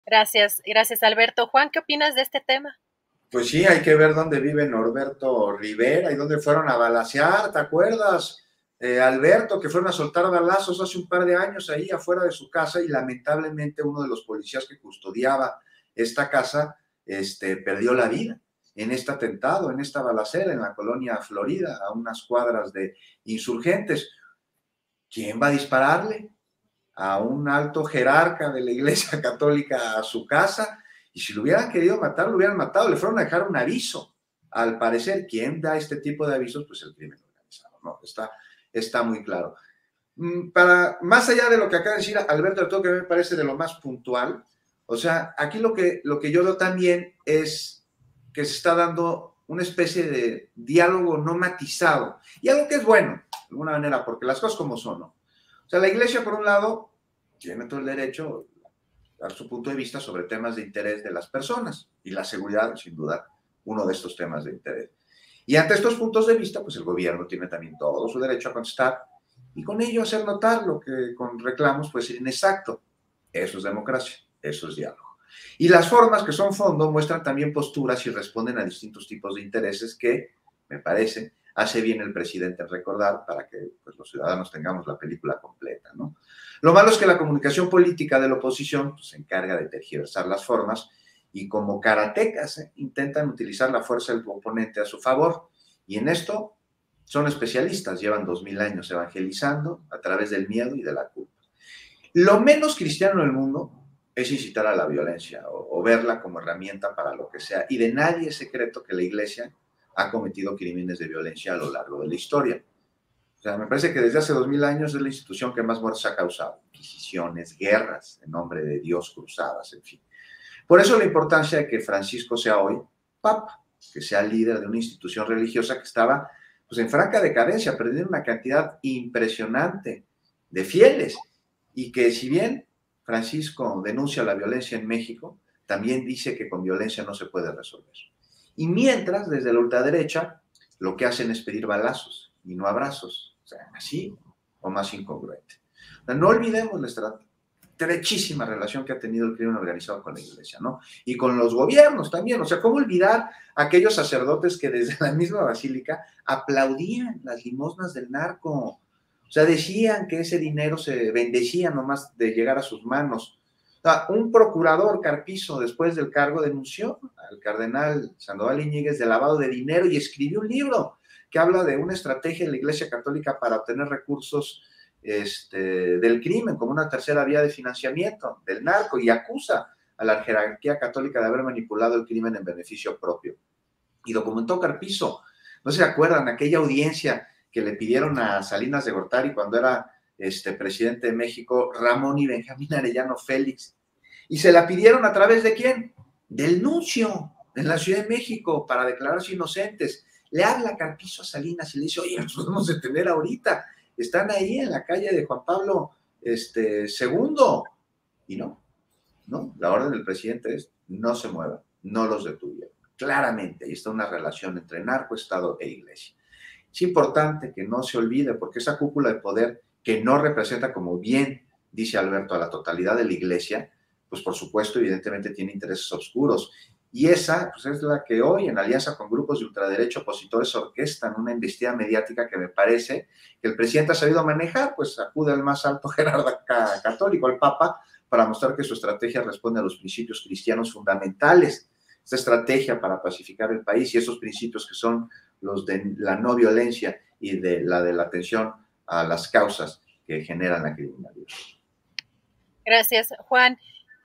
Iglesia orquesta embestida mediática en alianza con ultraderecha opositora: Juan Becerra Acosta
Juan Becerra Acosta opina que la cúpula de la iglesia católica y grupos de ultraderecha opositora están orquestando una embestida mediática contra el gobierno de Andrés Manuel López Obrador.

Menciona algunos casos donde se evidenció la participación de jerarcas eclesiásticos en actos ilícitos.